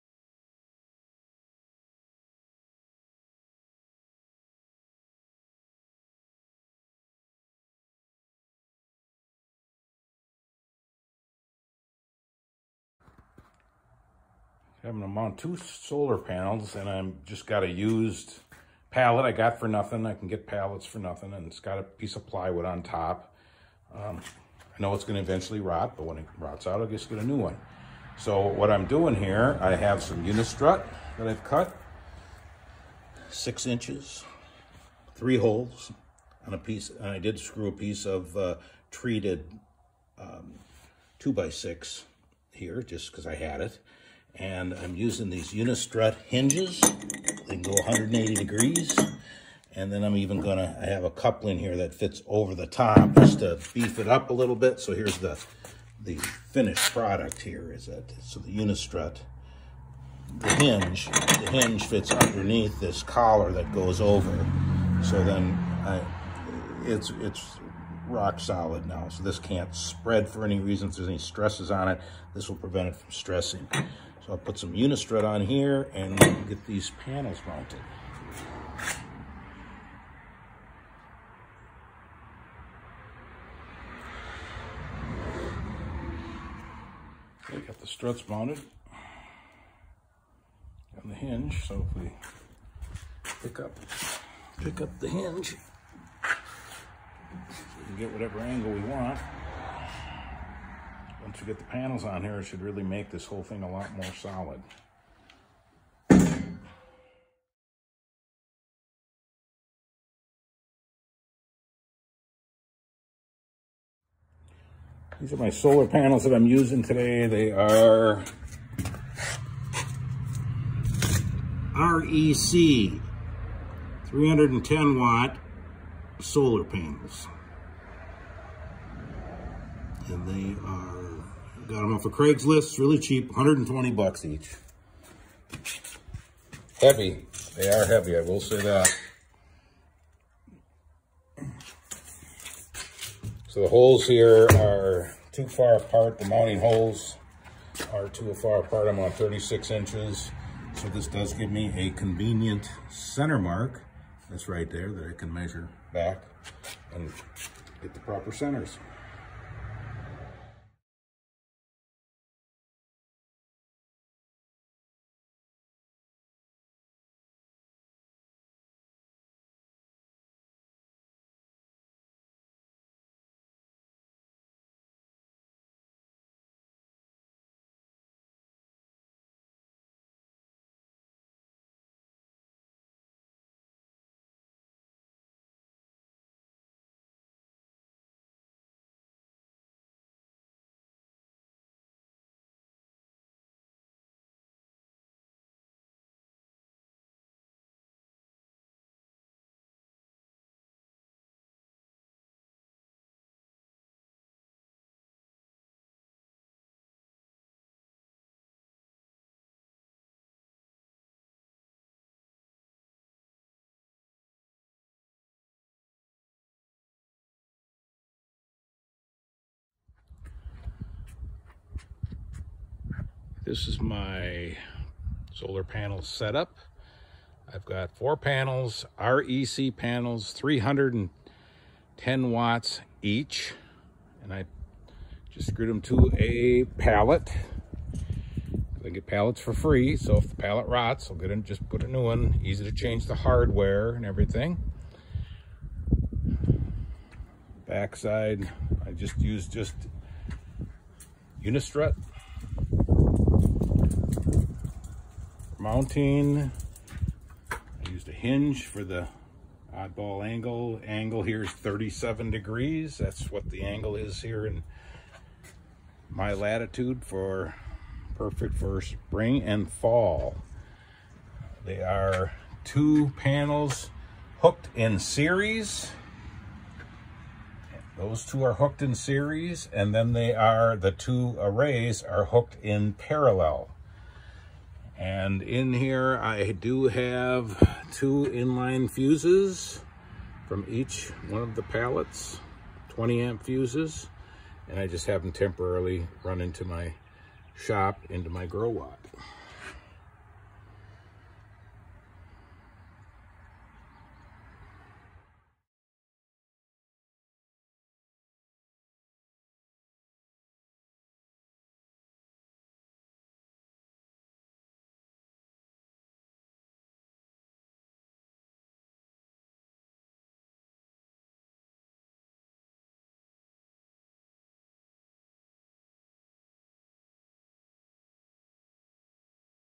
Growatt 3000 watt Solar Panel build part 3 - Cheapest way to mount solar panels
This is the end of my Growatt 3000 watt solar generator build series. Part 3. This is the cheapest way to mount solar panel. The solar panels were $120 each. With all the mounting hardware and the panels, total investment was $350 per pallet. The unistrut came from McMaster -Carr and Menards.